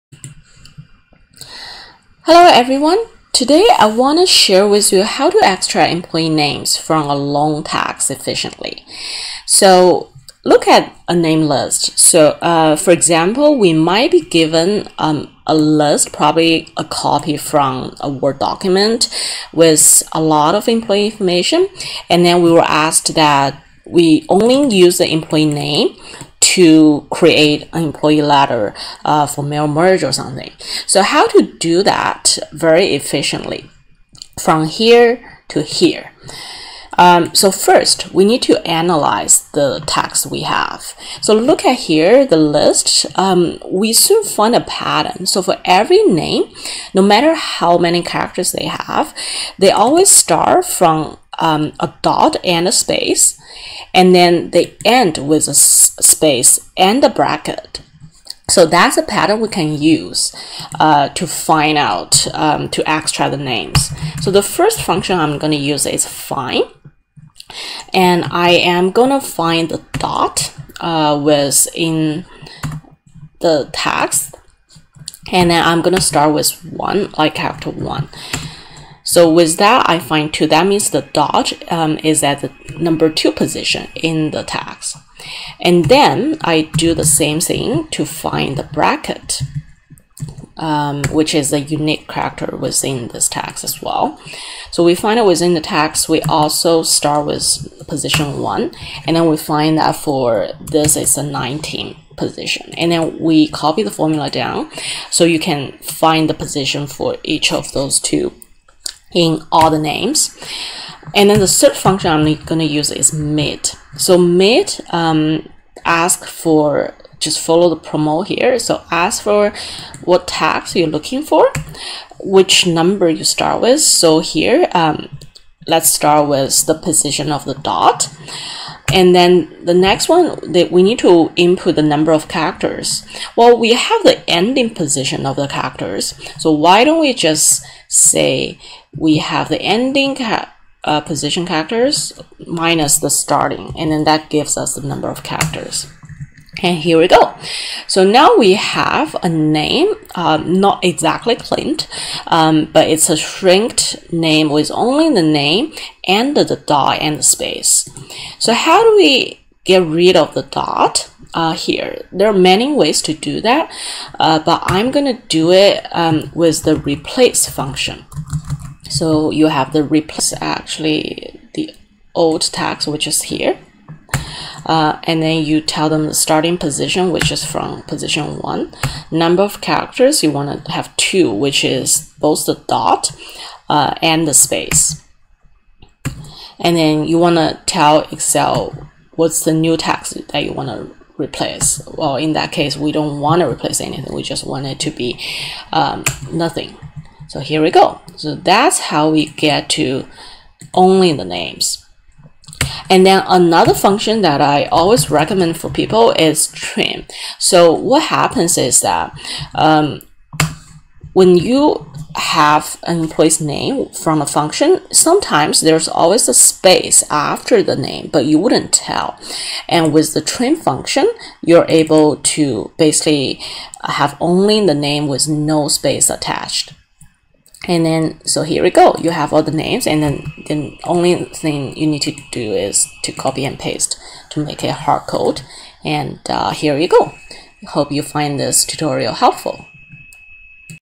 Excel for HR: Find and Replace Text in Employee Name
Business Scenario: How to extract Employee Names from a long text efficiently
Excel Function: Find(), Mid(), Replace(), Trim()